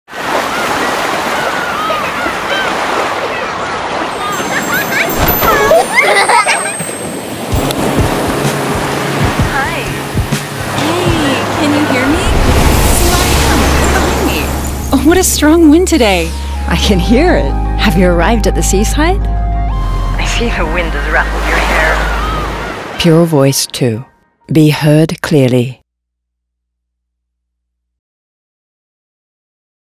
HUAWEI FreeBuds Pro 3 | Pure Voice 2.0
Experience the crystal-clear difference with the latest HUAWEI FreeBuds Pro 3.
Featuring Pure Voice 2.0, bid farewell to call disruptions in noisy environment, whether you're catching up with loved ones or on an adventure. Enjoy an 80% improved wind noise cancellation for smooth conversations by the sea or on a bike.